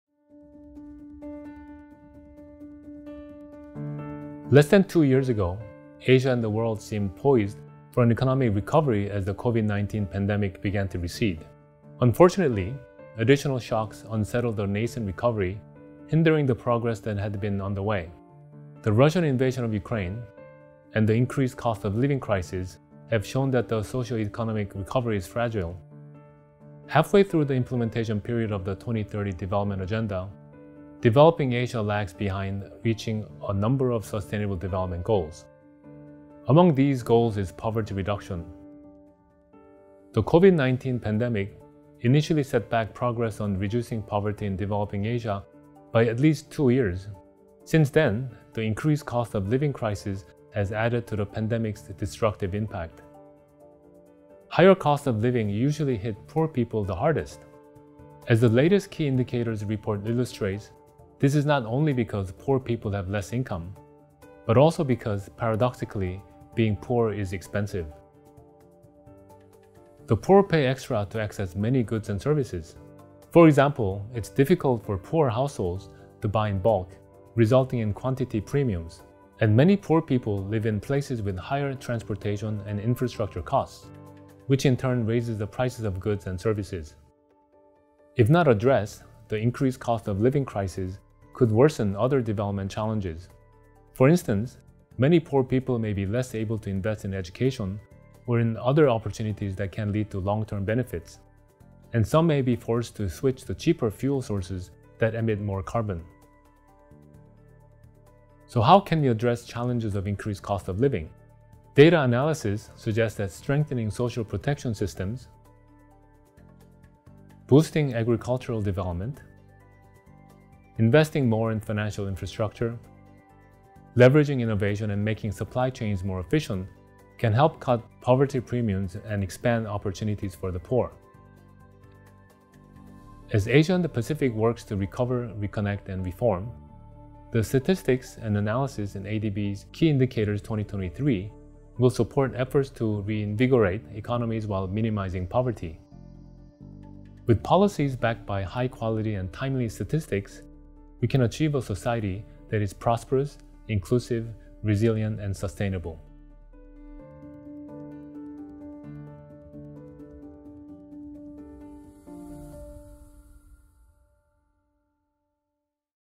Key Indicators for Asia and the Pacific 2023
Less than two years ago, Asia and the world seemed poised for an economic recovery as the COVID-19 pandemic began to recede. Unfortunately, additional shocks unsettled the nascent recovery, hindering the progress that had been under way. The Russian invasion of Ukraine, and the increased cost of living crisis, have shown that the socioeconomic recovery is fragile. Halfway through the implementation period of the 2030 Development Agenda, developing Asia lags behind in reaching a number of Sustainable Development Goals. Among these goals is poverty reduction.